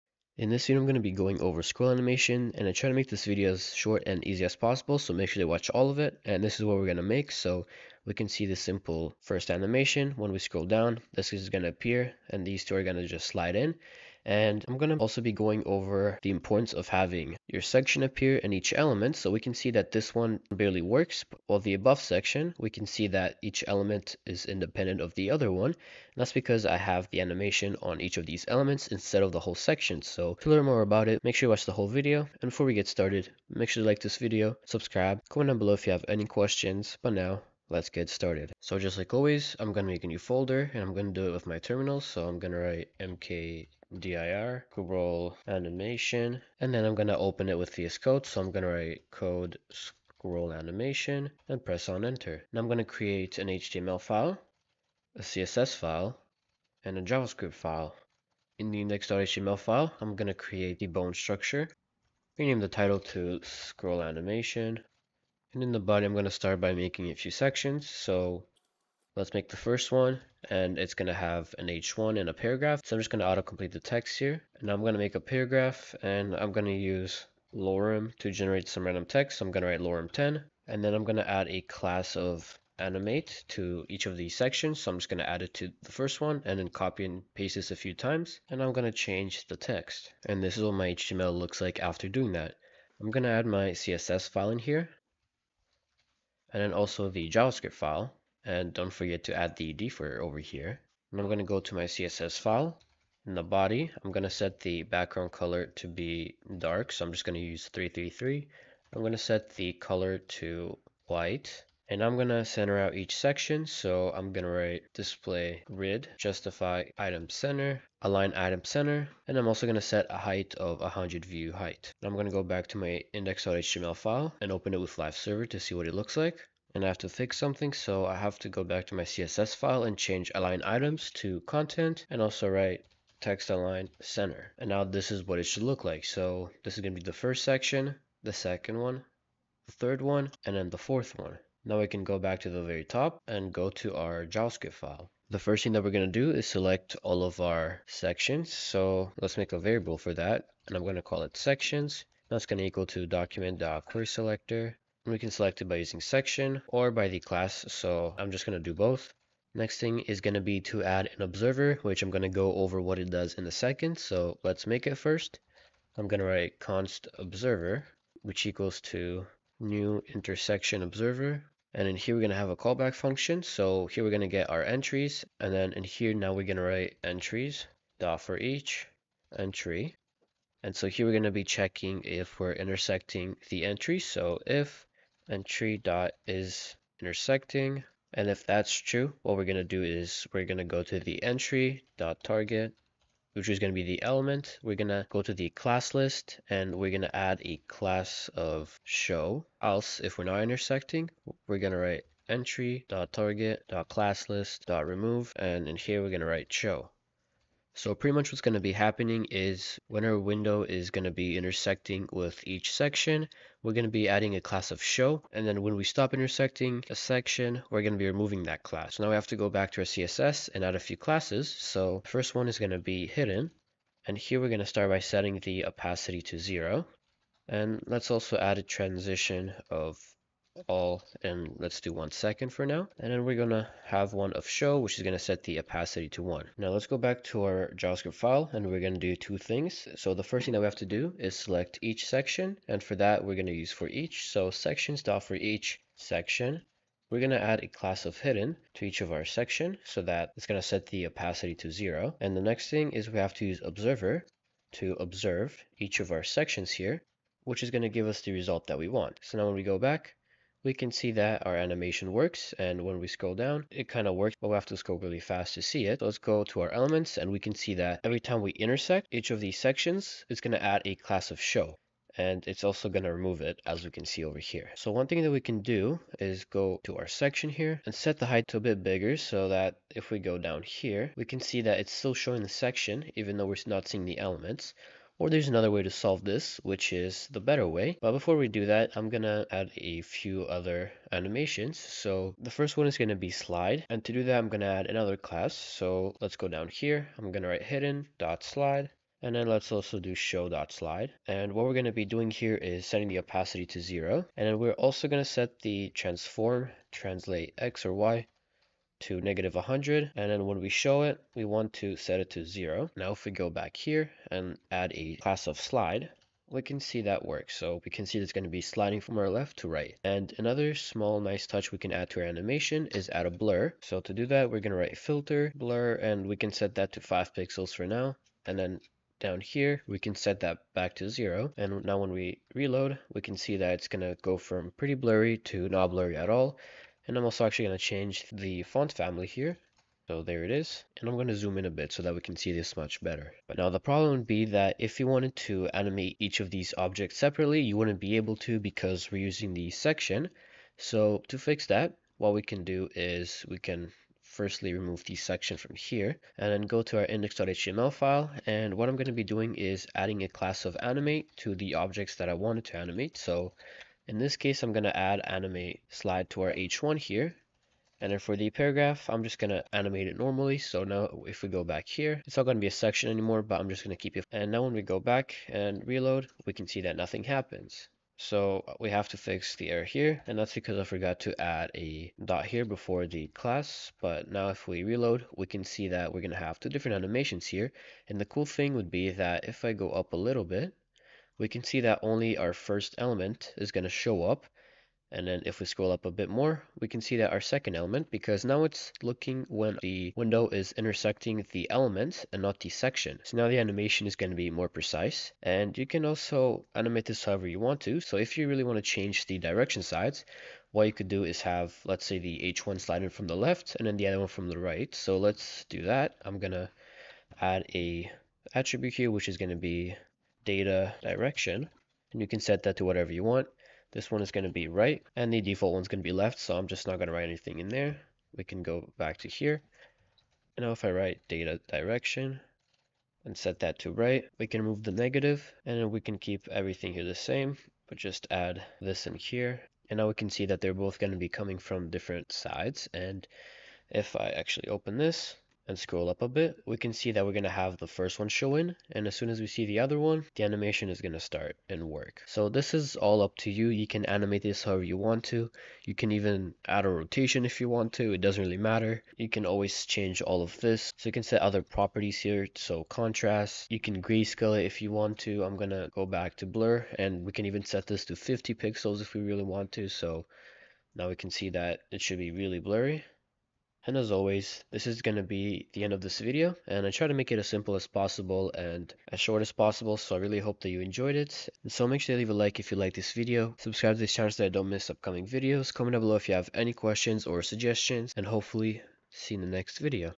Scroll Animation Made Easy: Intersection Observer API Tutorial
Discover the power of the Intersection Observer API by making an on scroll animation using HTML, CSS, and JavaScript!

⏳TIMESTAMPS⌛
0:00 Intro
0:38 Setup
0:58 HTML
1:40 CSS
2:23 JavaScript
3:38 Special Classes
3:56 JavaScript
4:47 Animations
5:40 Animation Fix
6:45 Slide Direction
7:32 Outro